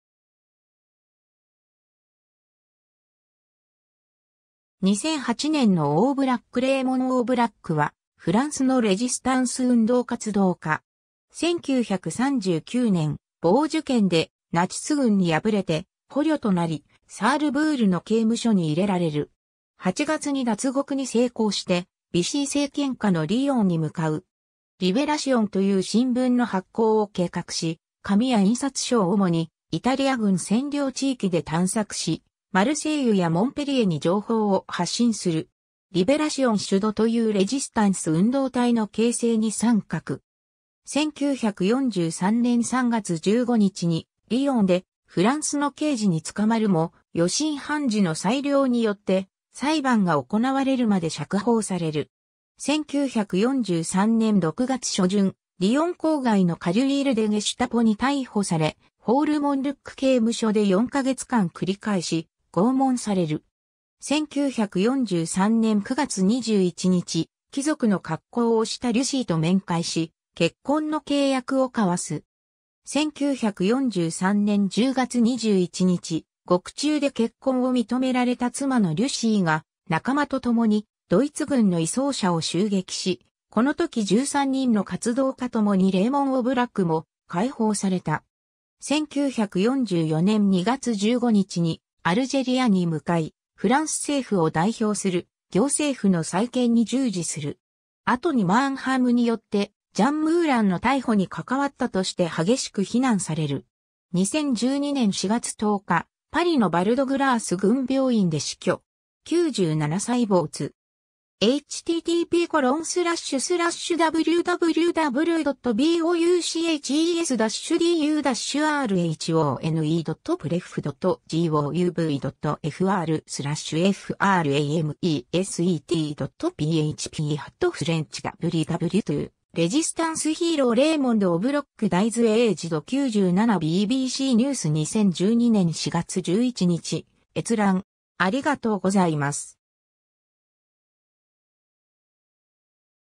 レーモン・オーブラック
2008年のオーブラック レーモン・オーブラック（フランス語: Raymond Aubrac、本名 Raymond Samuel、1914年7月31日 - 2012年4月10日）は、フランスのレジスタンス運動活動家。
 1939年、ヴォージュ県でナチス軍に敗れて捕虜となり、サールブールの刑務所に入れられる。
8月に脱獄に成功して、ヴィシー政権下のリヨンに向かう。
 リベラシオンという新聞の発行を計画し、紙や印刷所を主にイタリア軍占領地域で探索し、マルセイユやモンペリエに情報を発信する。
 リベラシオン・シュドというレジスタンス運動体の形成に参画。
1943年3月15日にリヨンでフランスの刑事に捕まるも、予審判事の裁量によって、裁判が行われるまで釈放される。
1943年6月初旬、リヨン郊外のカリュイールでゲシュタポに逮捕され、フォール・モンルック (Fort Montluc) 刑務所で4ヶ月間繰り返し拷問される。
1943年9月21日、貴族の格好をしたリュシーと面会し、結婚の契約を交わす。
 1943年10月21日、獄中で結婚を認められた妻のリュシーが仲間とともにドイツ軍の移送車を襲撃し、このとき13人の活動家ともにレーモン・オブラックも解放された。
1944年2月15日にアルジェリアに向かい、フランス政府を代表する行政府の再建に従事する。
後にマーンハムによってジャン・ムーランの逮捕に係わったとして激しく非難される。
 2012年4月10日、パリのバルドグラース軍病院（フランス語: Hôpital d'instruction des armées du Val-de-Grâce）で死去。
97歳没。
^ French WWII resistance hero Raymond Aubrac dies aged 97 BBC News 2012年4月11日閲覧